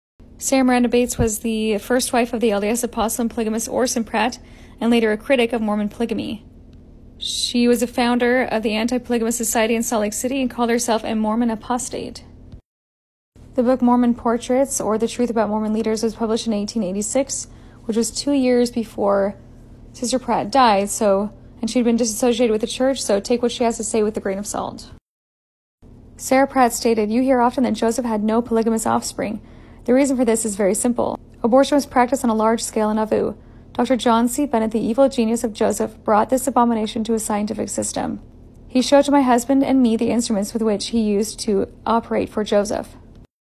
Nauvoo Abortions
Sarah Meranda Bates's story of Nauvoo abortions.